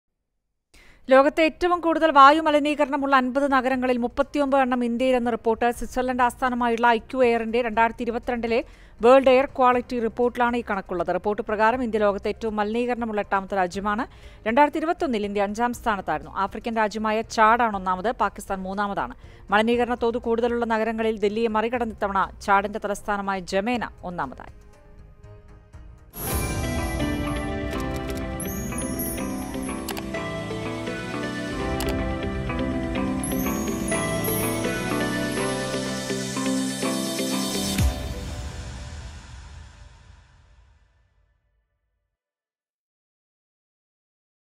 വായുമലിനീകരണത്തിൽ മുന്നിൽ ഇന്ത്യ | Air Pollution
വായുമലിനീകരണം കൂടിയ 50 നഗരങ്ങളിൽ 39 എണ്ണം ഇന്ത്യയിൽ